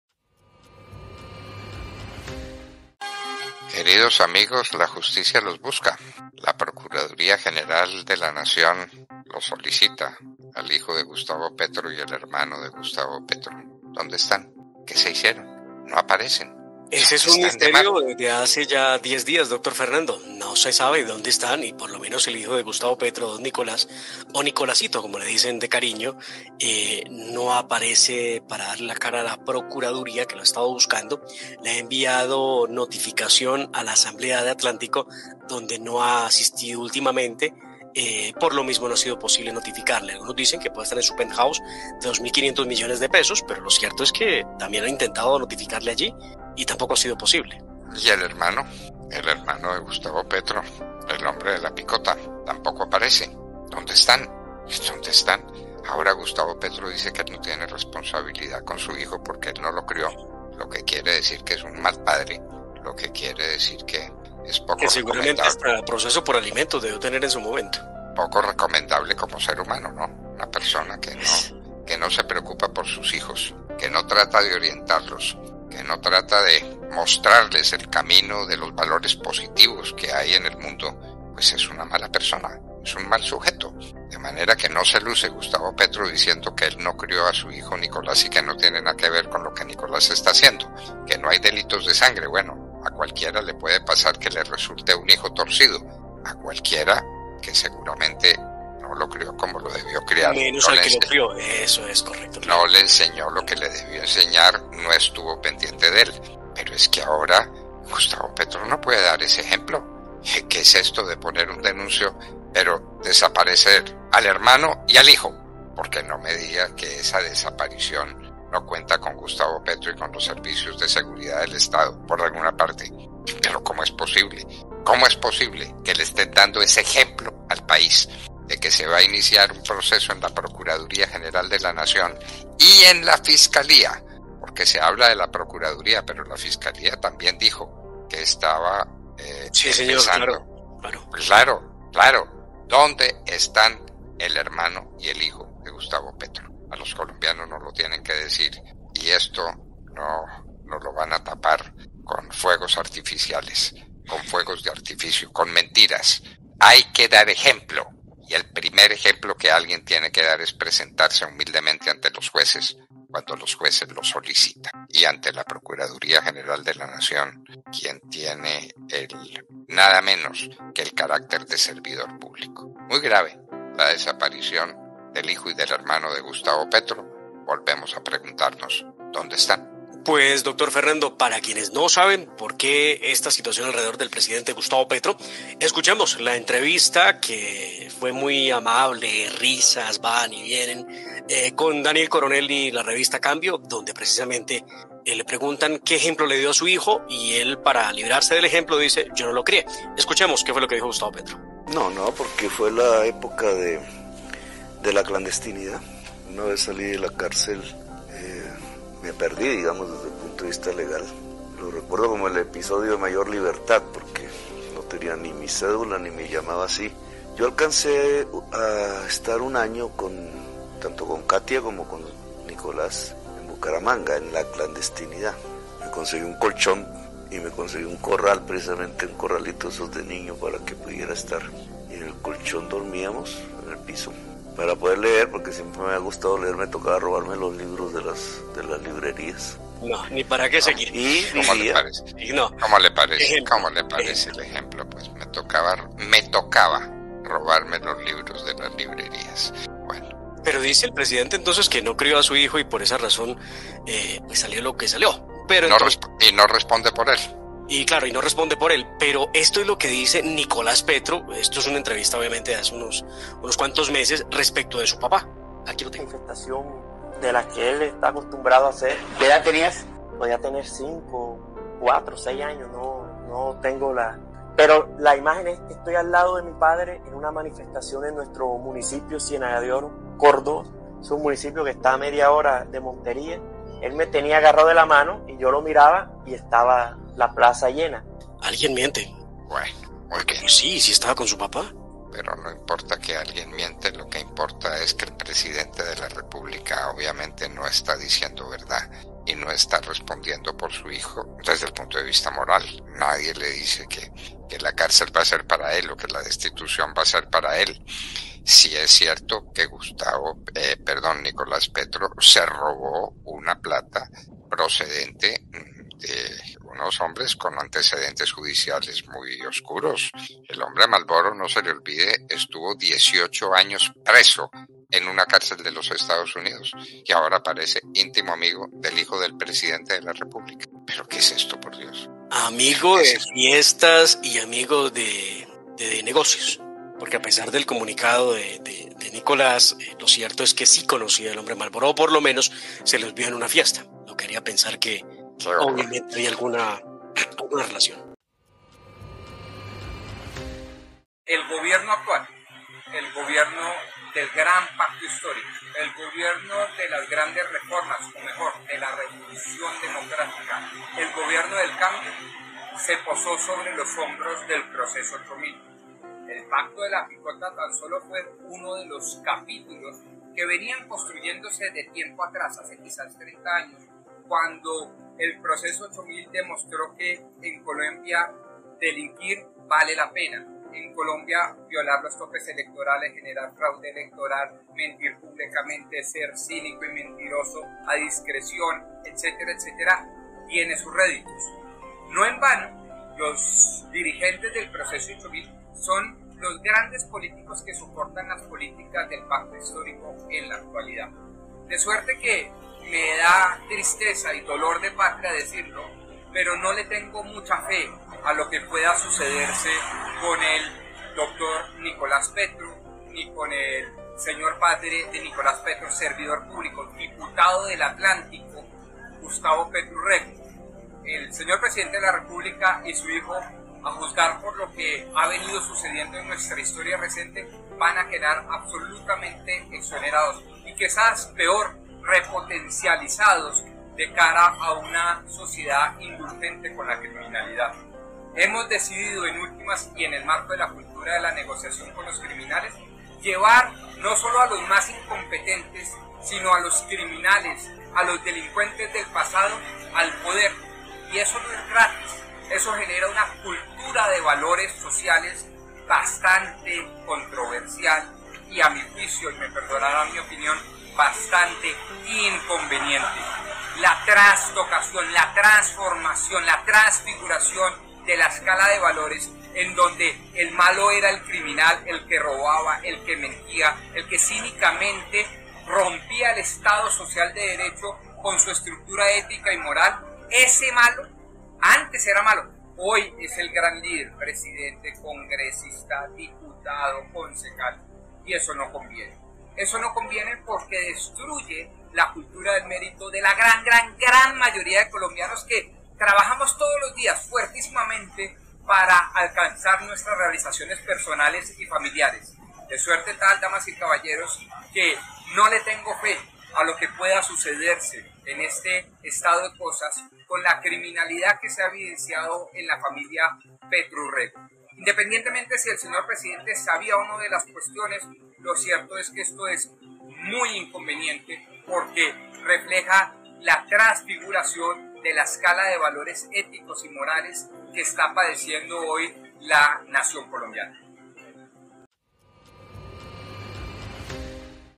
NO APARECEN FAMILIARES DEL PRESIDENTE PETRO 📢 Un Nuevo Proceso 8000
"La justicia colombiana los buscan ¿Dónde están? ni el hijo ni el hermano del presidente gustavopetro . ¿Alguien sabe de ellos? ¿Se fugaron?"
- La Hora de la Verdad -
El gobierno del "cambio" se posó sobre los hombros del proceso 8.000. No en vano quienes estuvieron implicados en ese proceso, son quienes soportan y respaldan las políticas del Pacto Histórico en la actualidad.
Rodrigo Pombo Cajiao